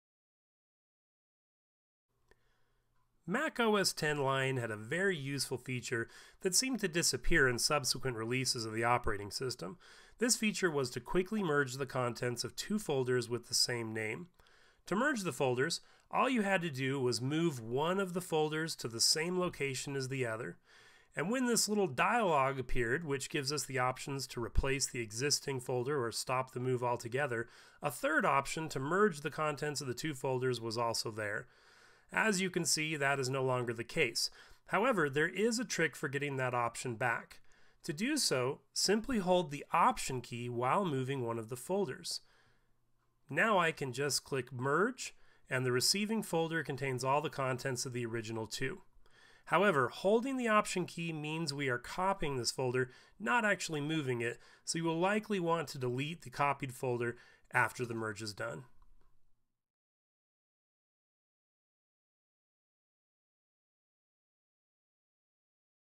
Merging Folders in OS X Mavericks
Learn how to regain the Merge option that went missing after OS X Lion.